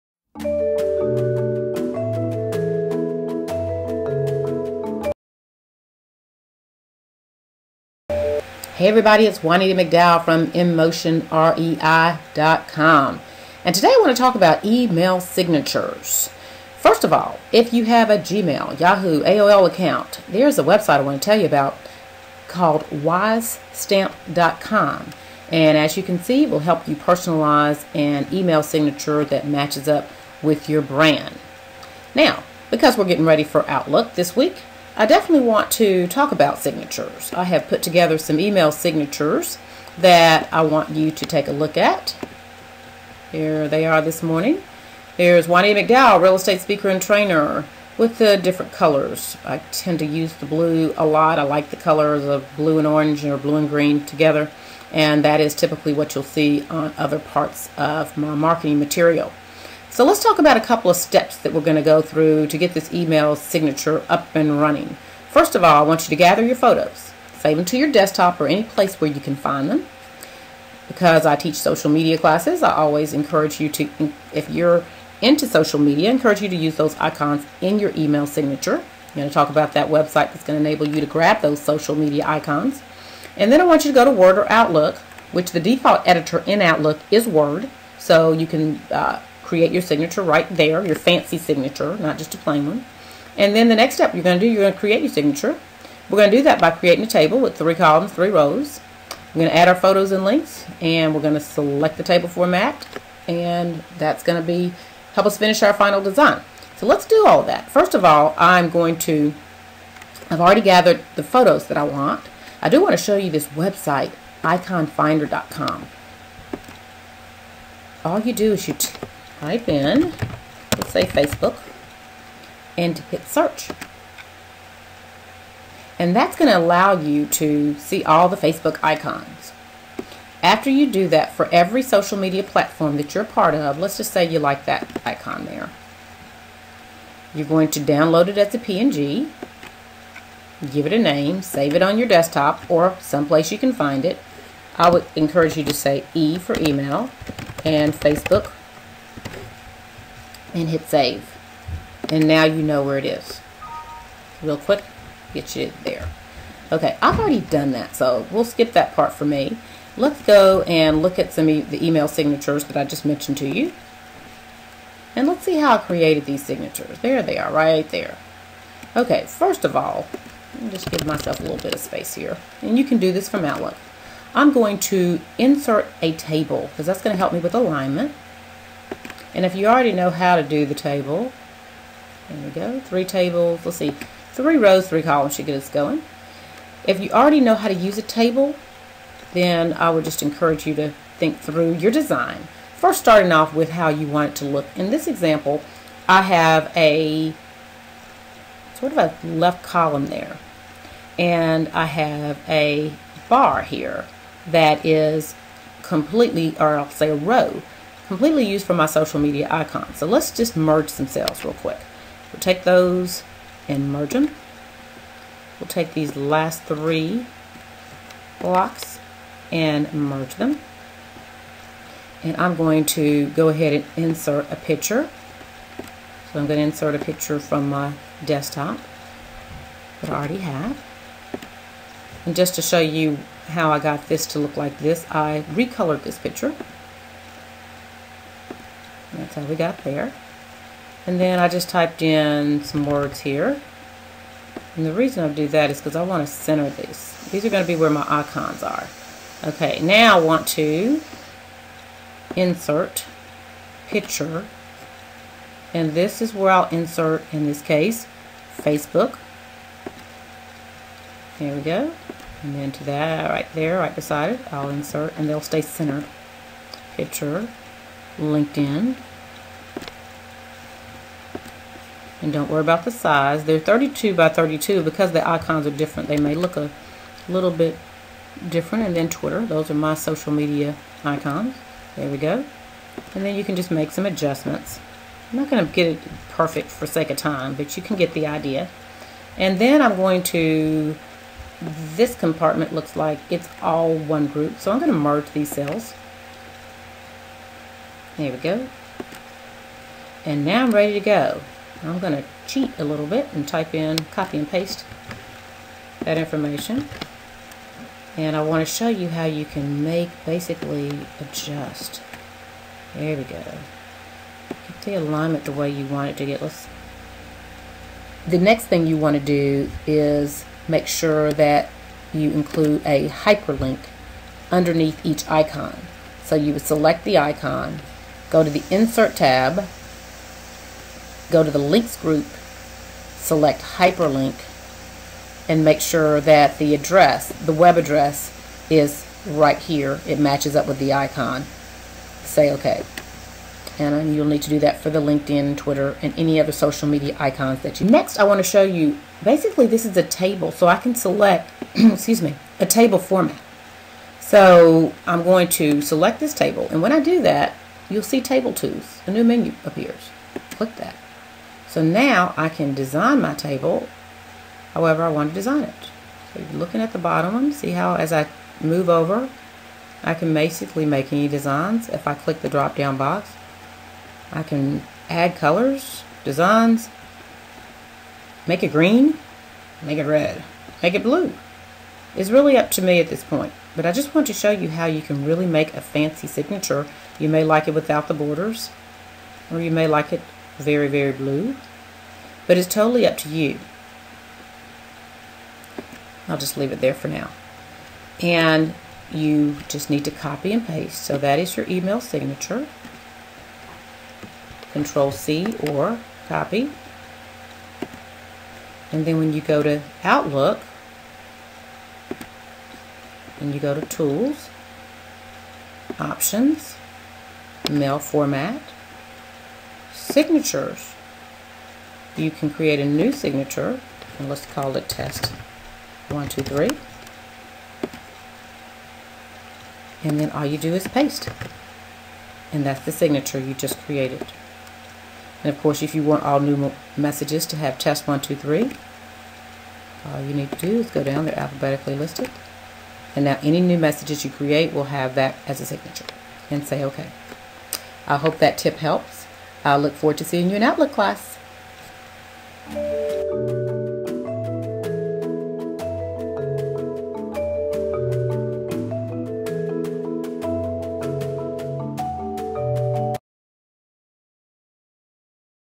Create a Fancy Email Signature with Social Media
| How to create a fancy email signature in Outlook using Social Media icons. There may be a small skip or 2 in this video. I had to go back and add the importance of including your hyperlinks underneath the icons.